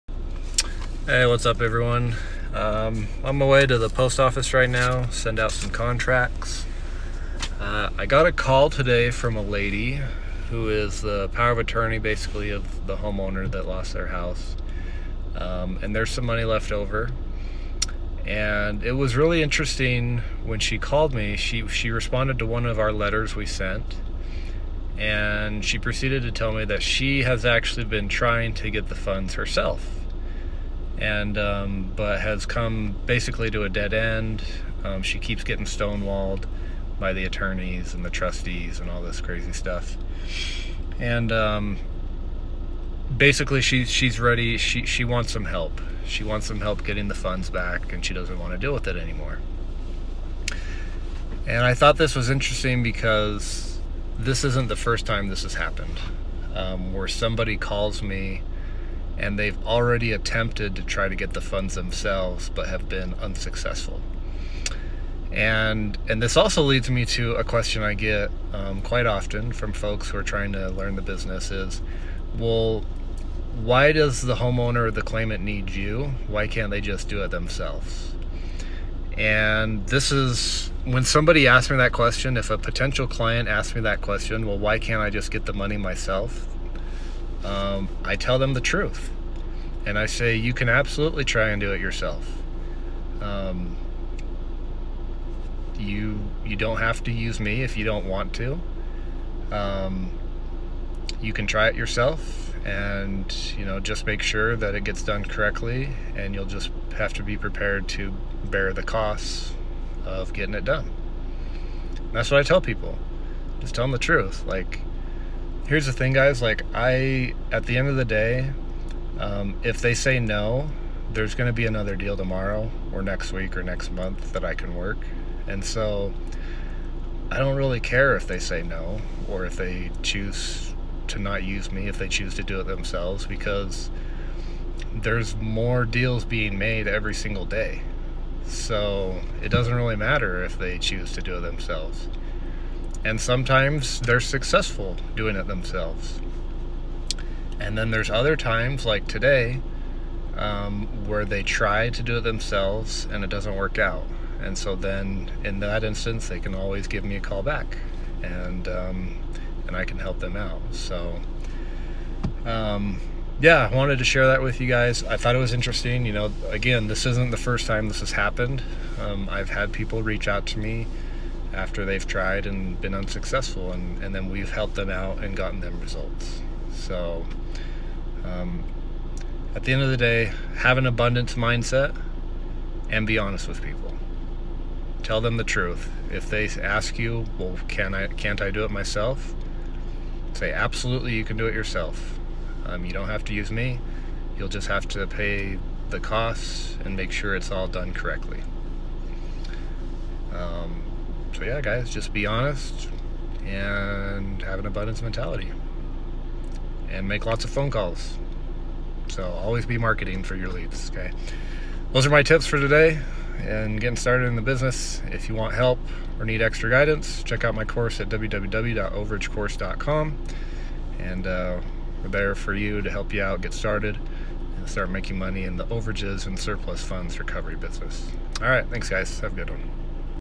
Why Can’t Someone Just Claim Overages/Surplus Funds For Themselves?
Today I answer a very common question that I get asked frequently.

“Why can’t someone just claim the overage or surplus funds for themselves, why do they need you to help them?”

Here is my response in this video.

For help getting started in surplus funds, and tax sale overage recovery business visit: